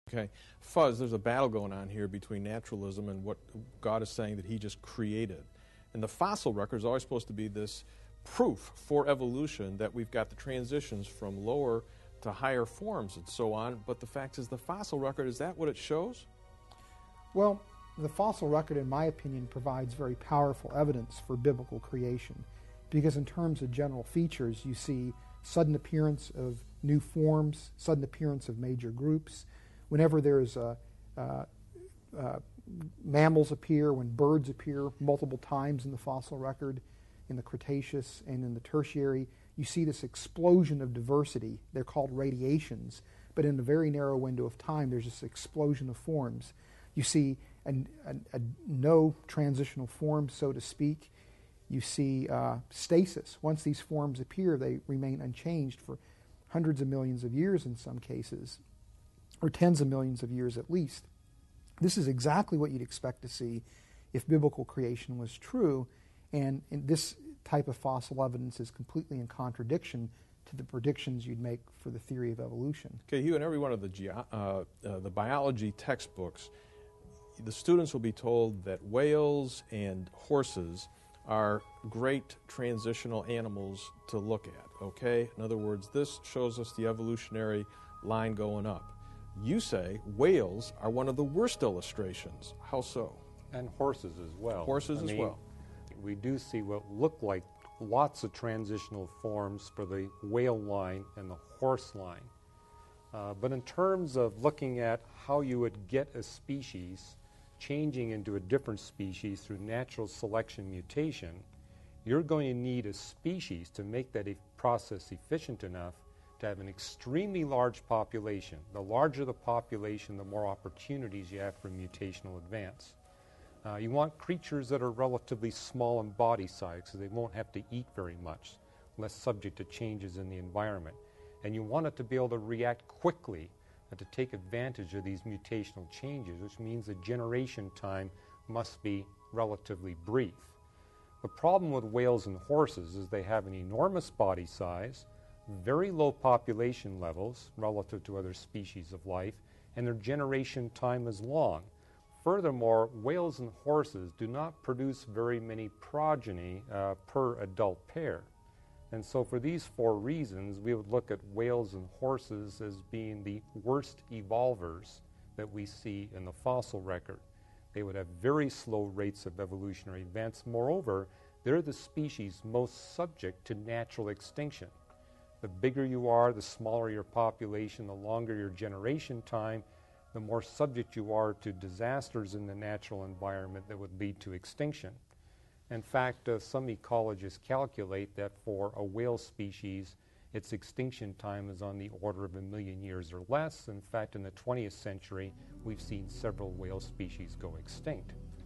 Does the fossil record show proof for evolution?
From our series entitled, "The Biblical Account of Creation: Does It Square With Scientific Evidence Today?", This clip discusses the question: Does the fossil record show proof for evolution?